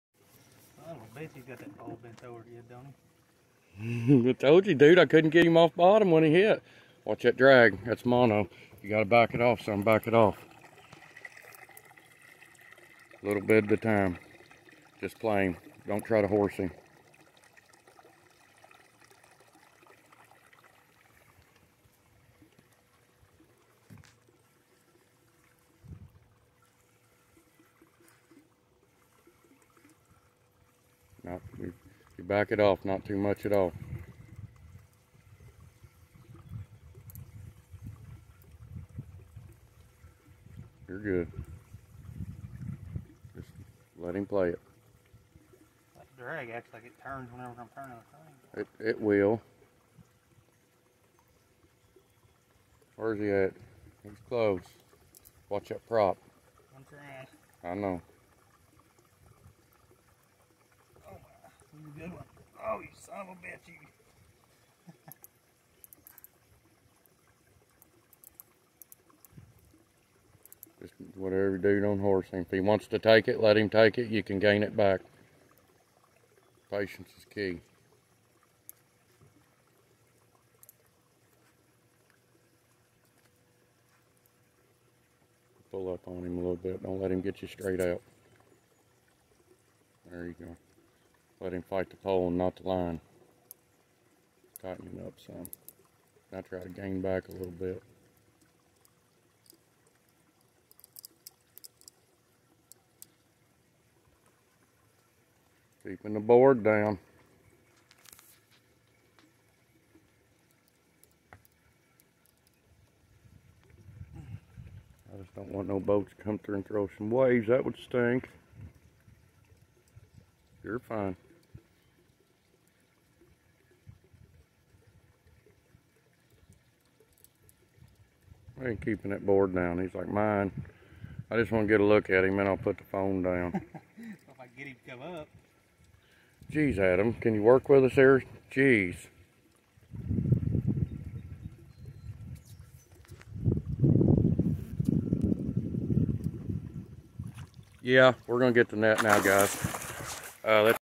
MONSTER 67 POUND BLUECATFISH!
Adam fighting a 67 pnd blue catfish